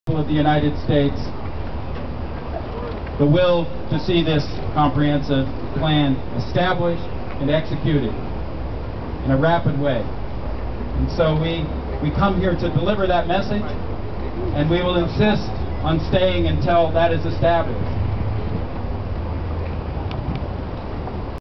Ken.Butigan.at.the.White.House (DC, 9-21)
Ken Butigan of Pace e Bene at the White House.